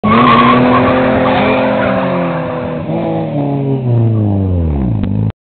Marce vs Torodespister
Tirada entre la TSX aspirada de Marcelo y la TSX turbinada de Torodespister, antras la Kangoo de Murphy y el Golcito que lamentablemente dijo basta.